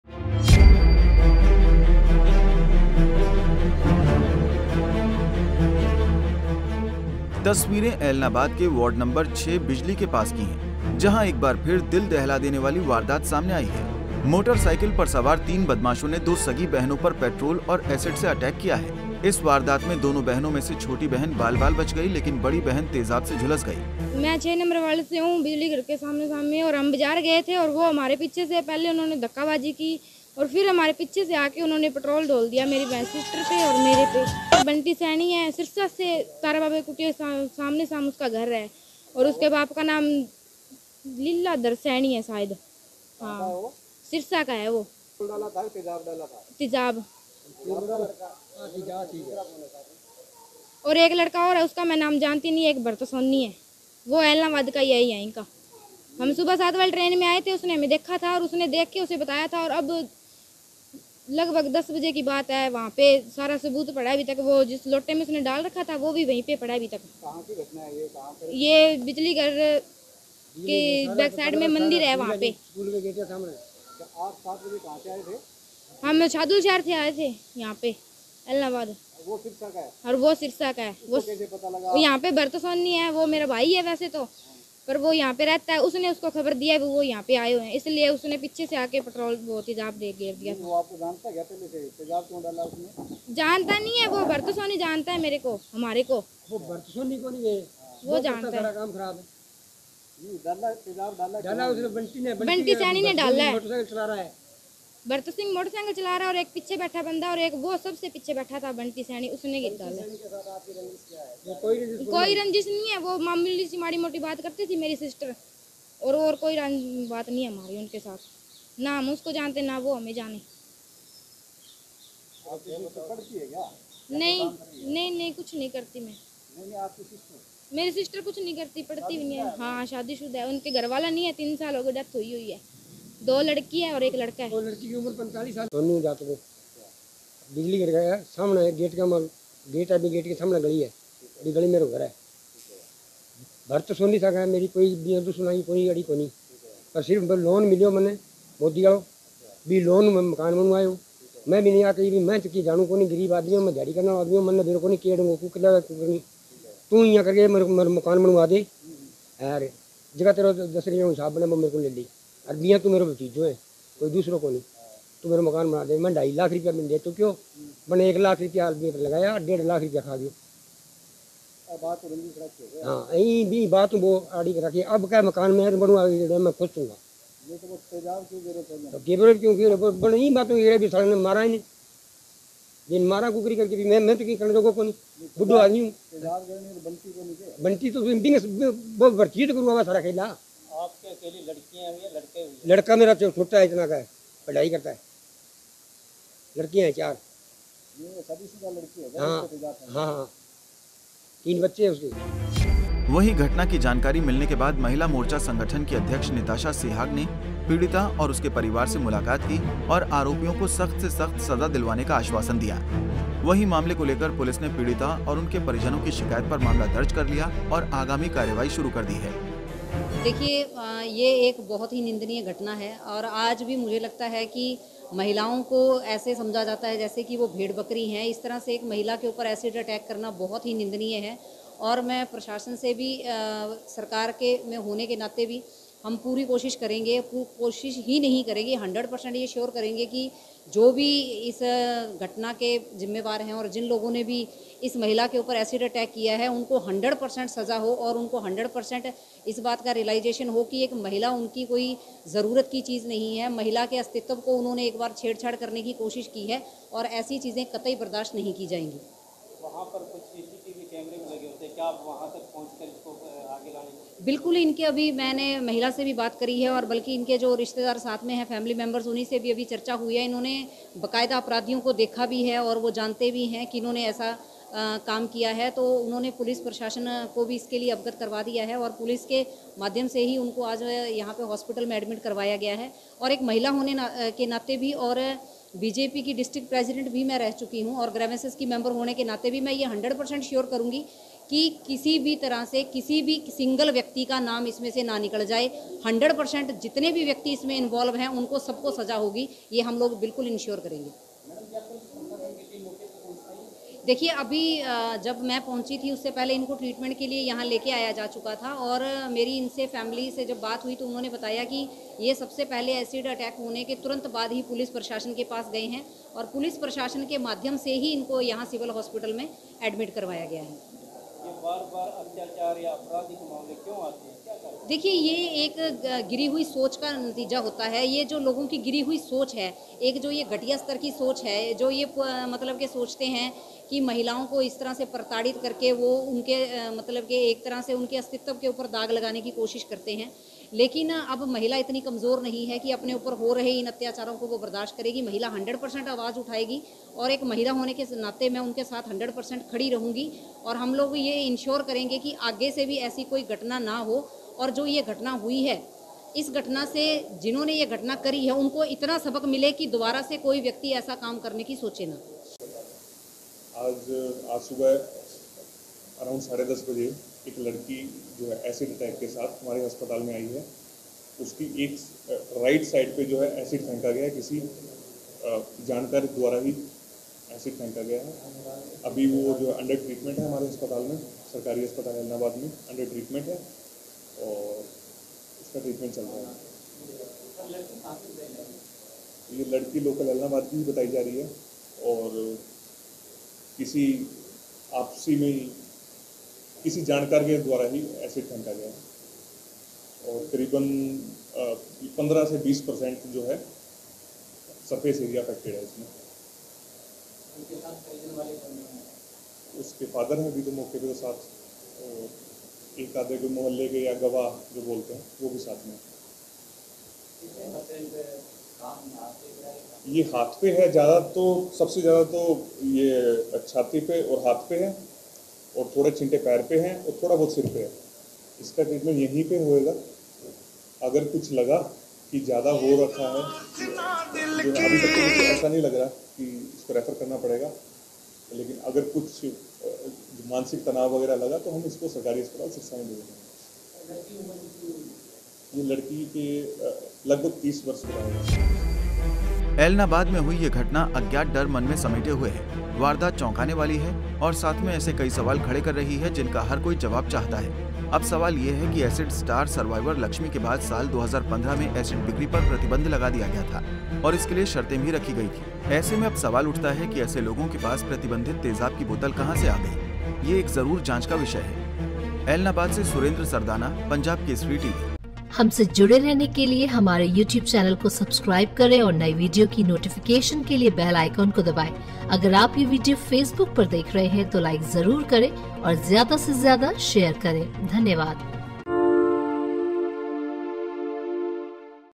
सादुलपुर की दो बहनों पर बदमाशों ने फेंका तेजाब, एक बाल बाल बची तो बड़ी बहन जुलसी
तस्वीरें ऐलनाबाद के वार्ड नम्बर 6 बिजली के पास की है जहां एक बार फिर दिल दहला देने वाली वारदात सामने आई है... मोटर साइकल पर सवार तीन बदमाशों ने दो सगी बहनों पर पैट्रोल और एसिड से अटैक किया है...इस वारदात में दोनों बहनों में से छोटी बहन बाल बाल बच गई लेकिन बड़ी बहन तेजाब से जुलस गई